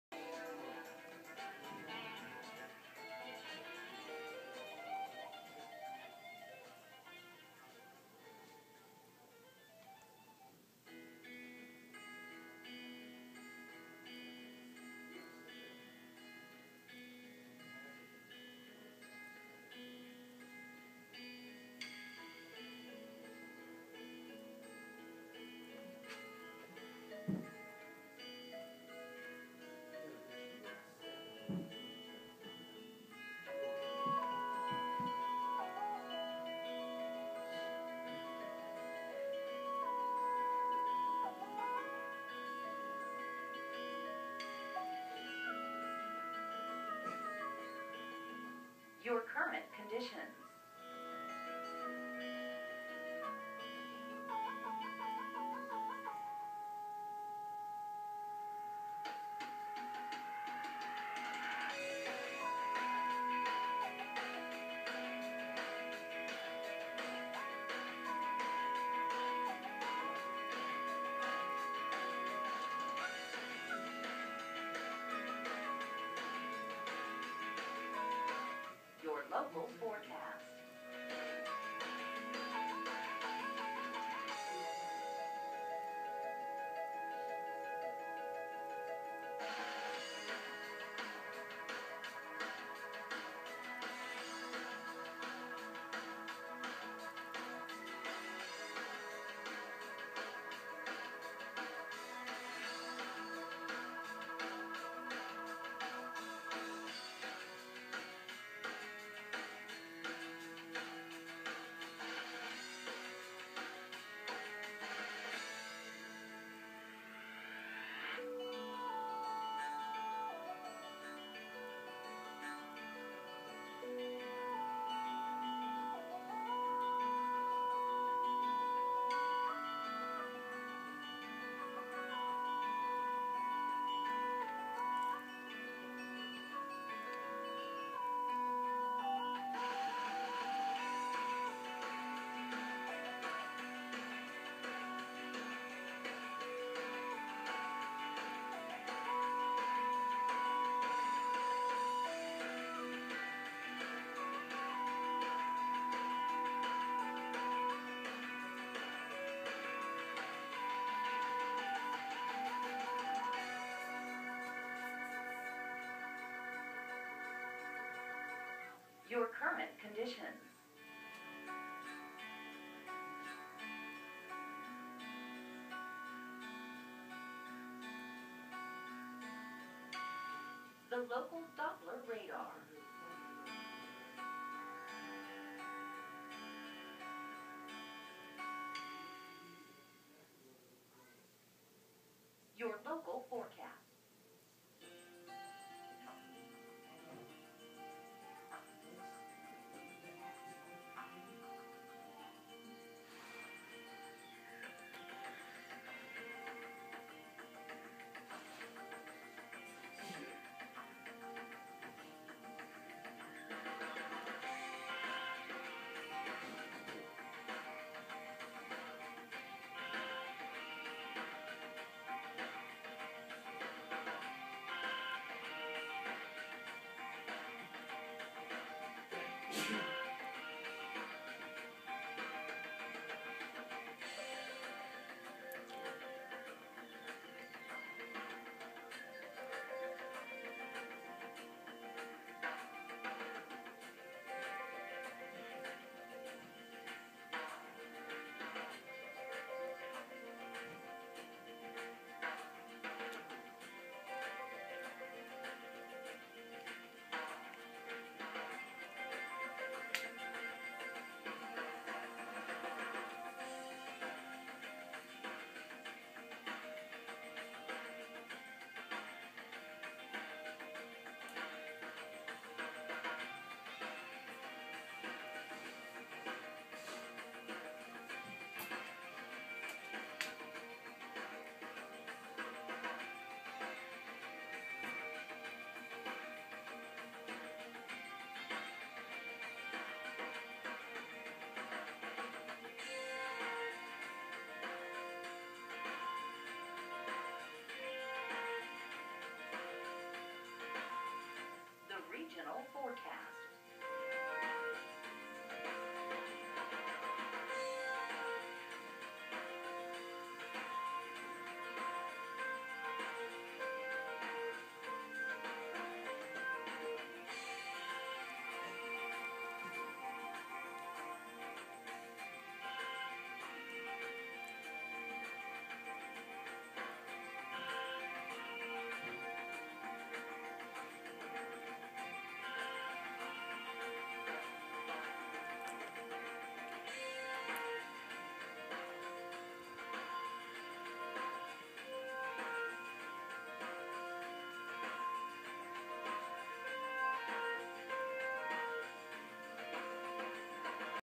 TWC Weatherscan: 4-21-12
A hot, humid Spring day turns into a round of thunderstorms.  We got a strong thunderstorm about an hour after this.  Sorry for the poor quality, I used my non-HD camera.

UNIT:  Weatherscan
REPORTING STATION:  Baltimore, MD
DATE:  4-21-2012
TIME:  6:10 p.m.
CONDITIONS:  Mostly Cloudy - 81